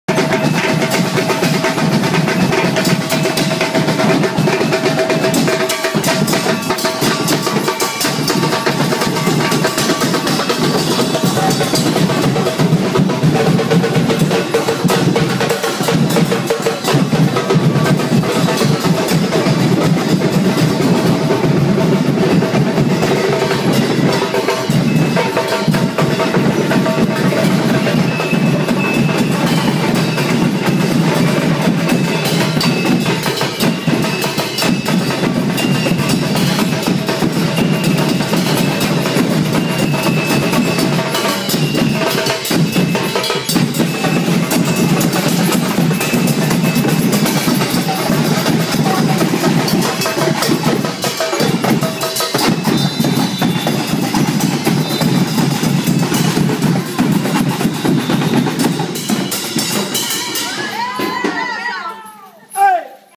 Escuela de Verano Cjex convivencia Carnavalera
Convivencia de jóvenes carnavaleros de Makumbas, Achikitú, montijanos, la kochera y vaivén en Barcarrota.